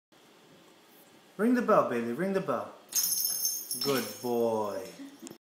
Ring The Bell Bailey
My 6 month old Yorkie learned how to ring a bell, so we can let him out to do his "business"